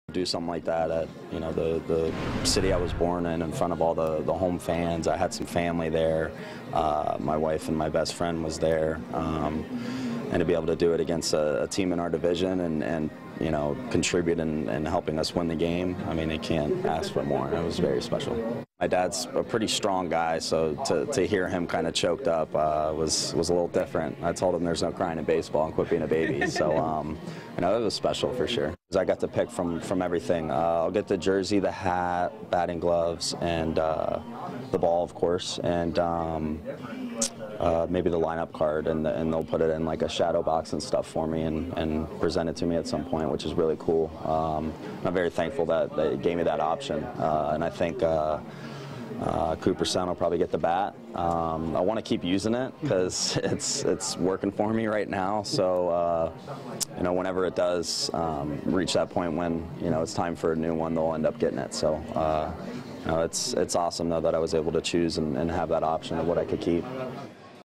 Scooter Gennett's special night capped off by father's tears
Gennett shares the emotions him and his family were feeling following his 4-HR night